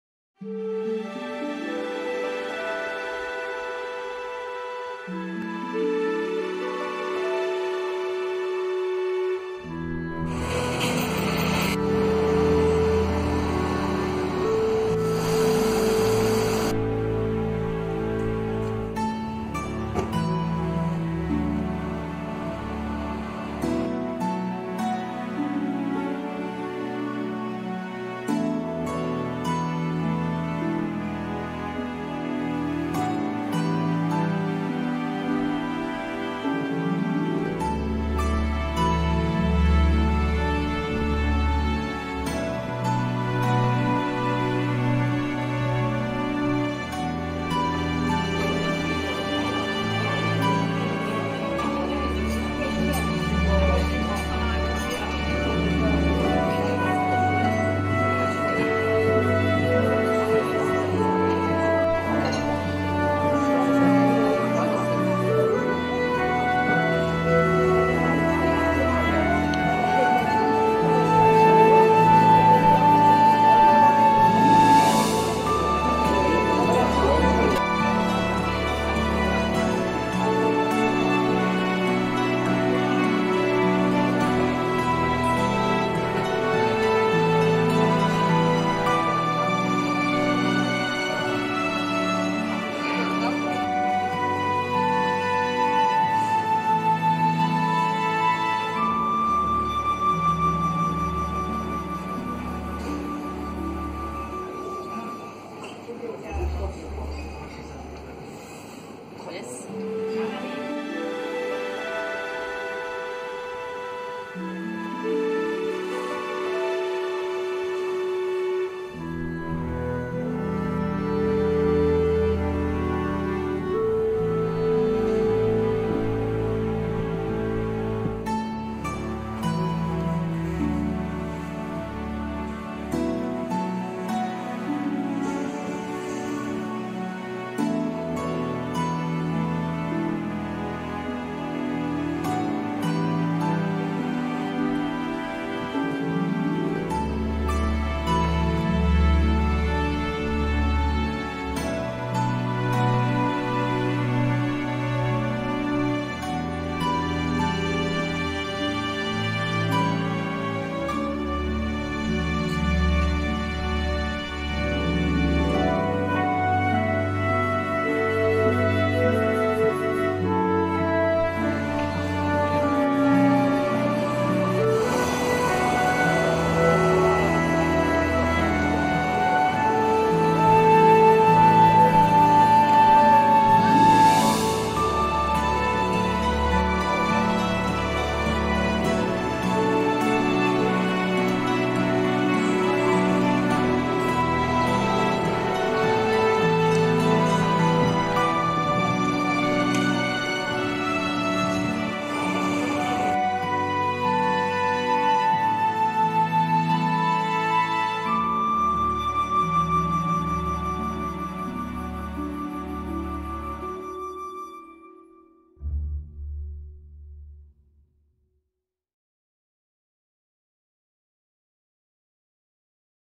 Walking in Troyes France
In this summer Holiday, I visited Champagne and Picardie France.
France is more than Paris. Troyes, Laon, Amiens, and other cities are waiting for you.